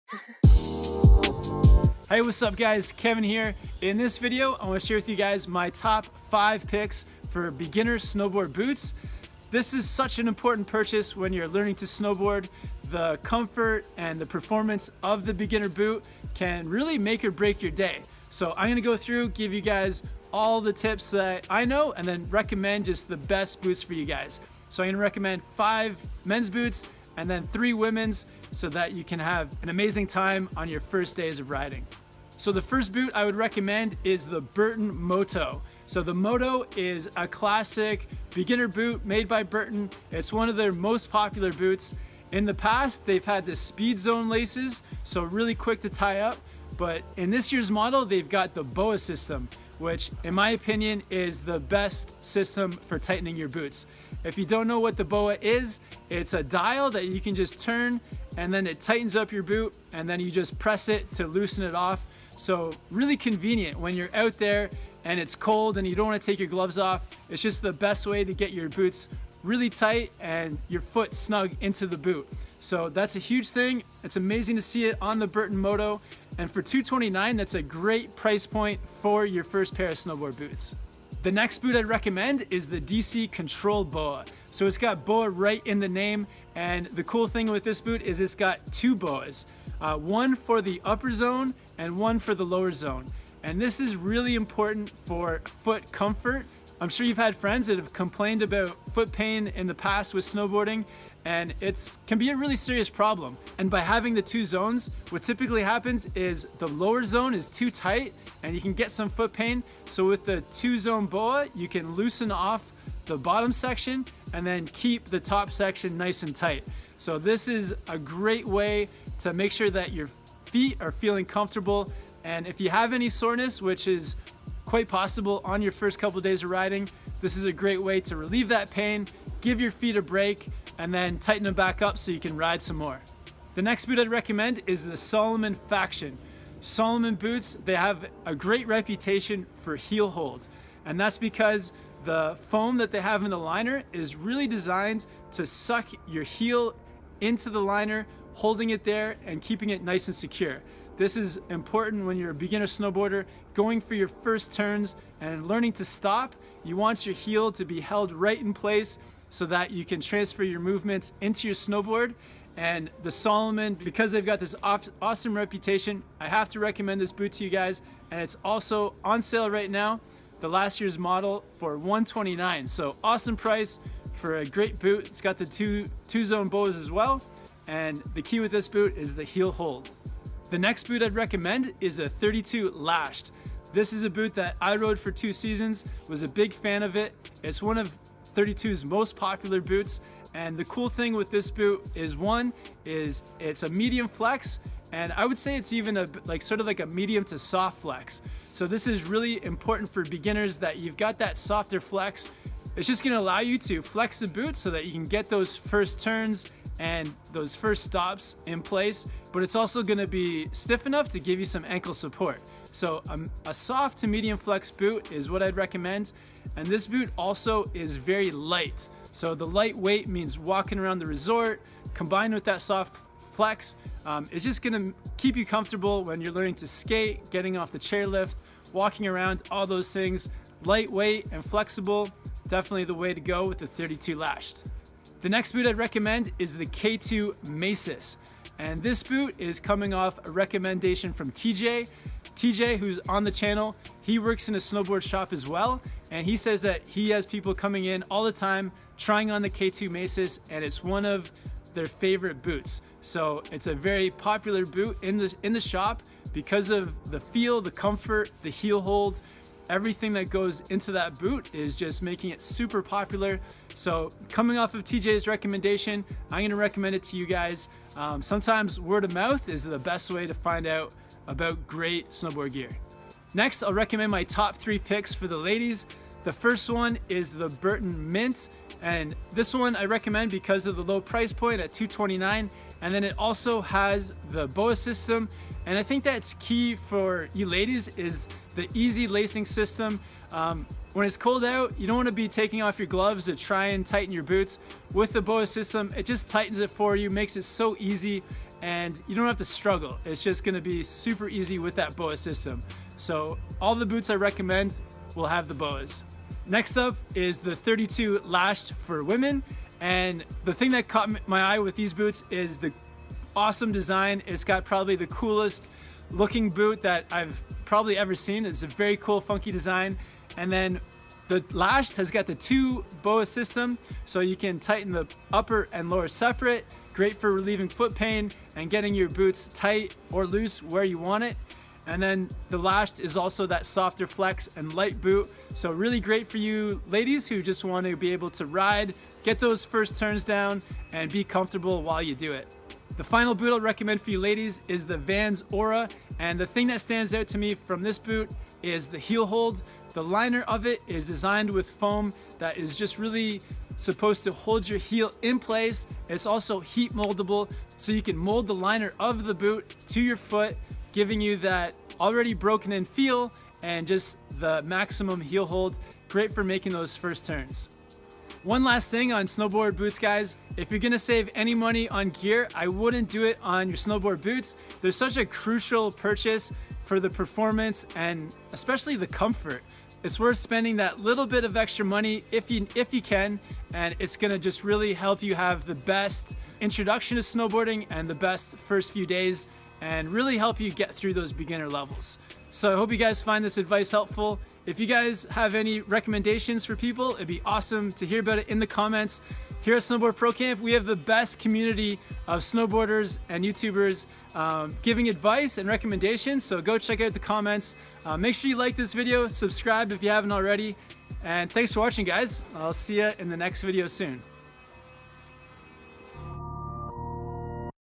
TOP 5 BEGINNER SNOWBOARD BOOTS
TOP 5 BEGINNER MEN'S BOOTS

In this video I want to share with you my top 5 picks for beginner snowboard boots. This is such an important purchase because the performance and comfort of the boot can really make or break your first day snowboarding. I'm going to give you guys the best tips I know for buying snowboard boots and give you my top 5 mens snowboard boots and top 3 womens snowboard boots. The first thing to consider when buying snowboard boots is the cost. For beginner boots find a pair on the lower end of the price scale. Next I highly recommend the the Boa system for tightening the boots. This is an easy dial that can quickly tighten your boots so you can avoid that hassle of traditional laces. I recommend buying snowboard boots that are a soft or medium flex. I also recommend finding boots with a liner that has strong heel hold. This will keep your heel in place when making turns and stopping. Check out the snowboard gear playlist for more gear tips!